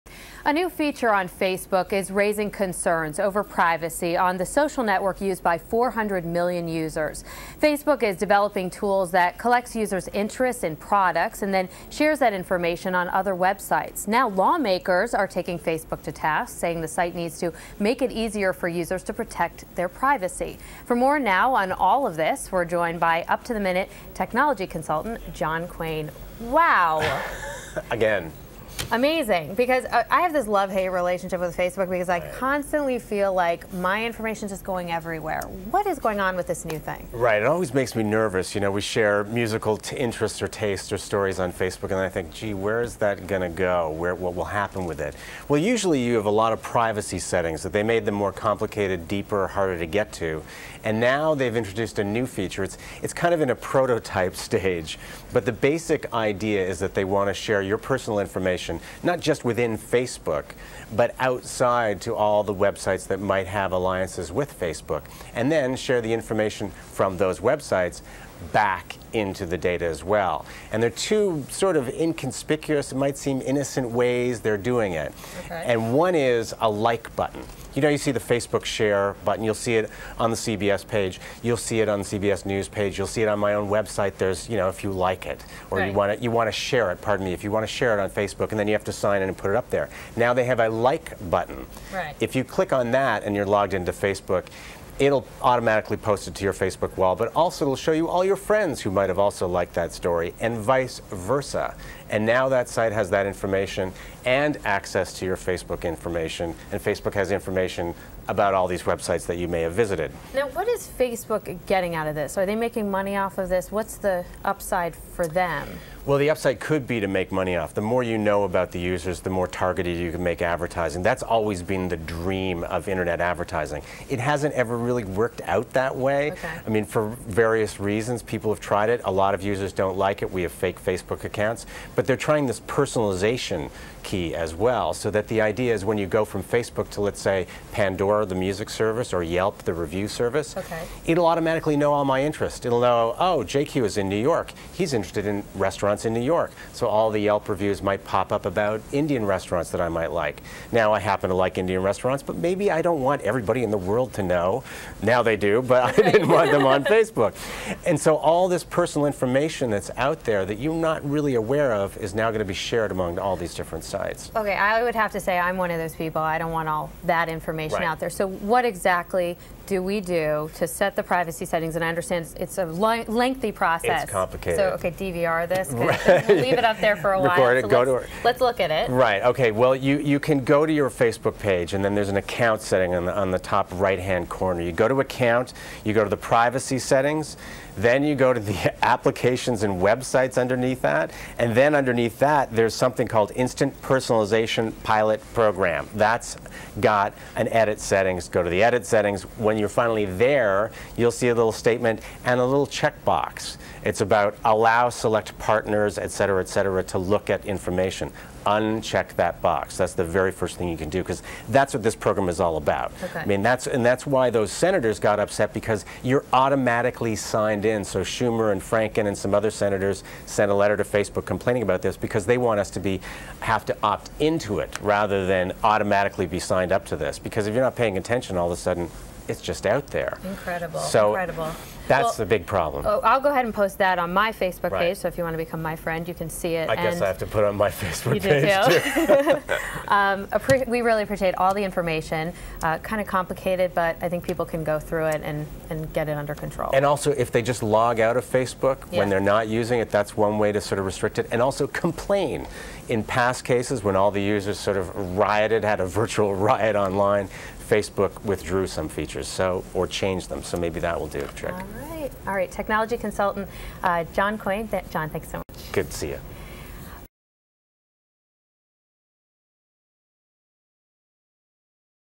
Facebook Privacy
Up to the Minute technology consultant John Quain speaks to Up To The Minute anchor Michelle Gielan about Facebook's new privacy feature and steps you should take if you want to limit personal information shared on the social networking site.